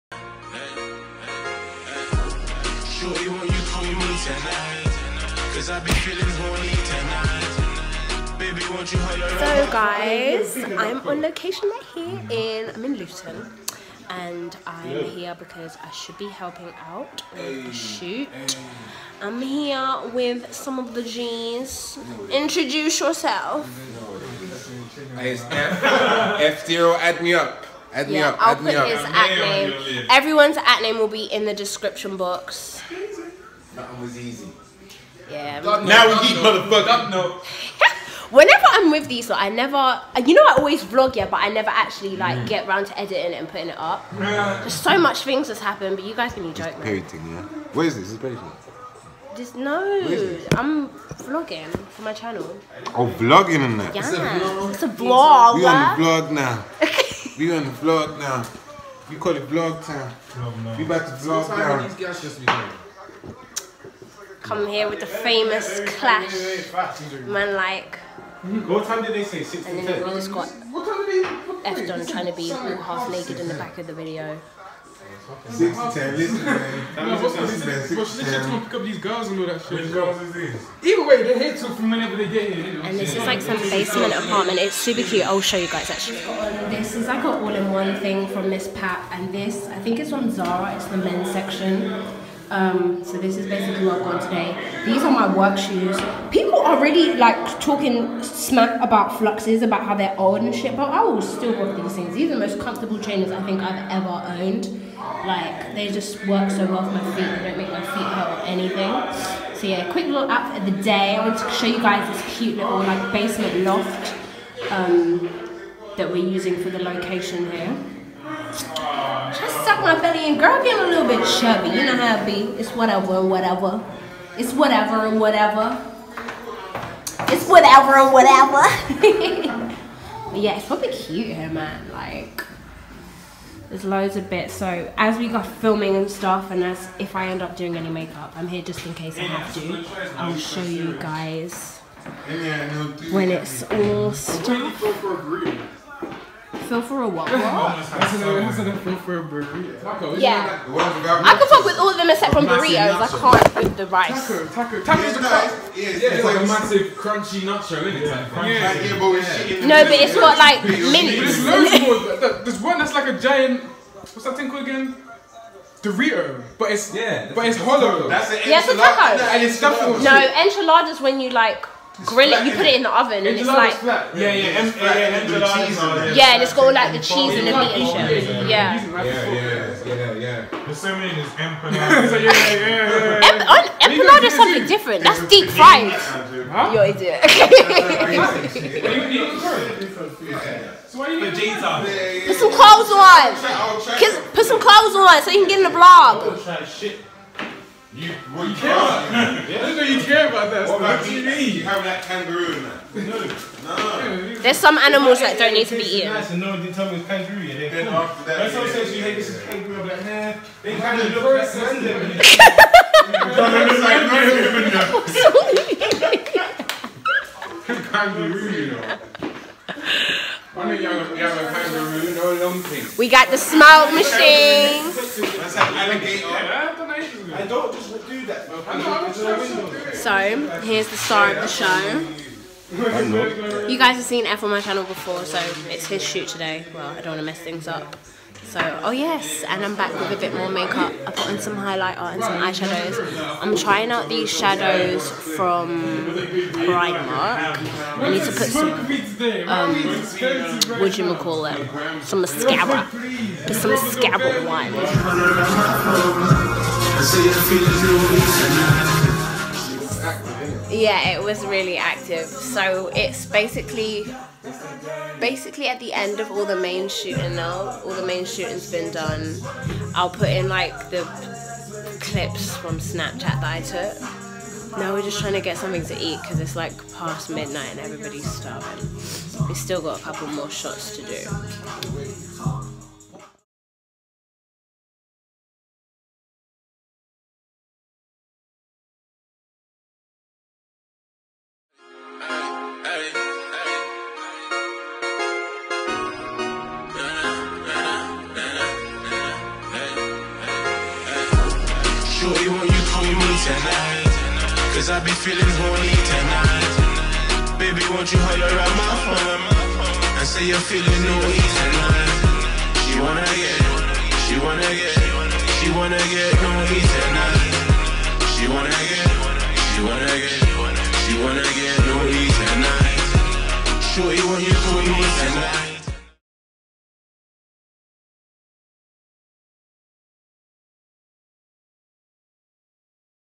A VLOG | Naughty Tonight
[EXPLICIT CONTENT VEWERS DISCRETION IS ADVISED]

Wassup Sqwaard!
So I thought I'd try to actually upload this blog for a change. As many of you may know I am a Makeup Artist and I have worked on a couple of video shoots but I always have the most fun when I'm working these these guys so I thought I'd share it with you all. I know I'm not the best blogger ion the world but hopefully you guys find this video entertaining.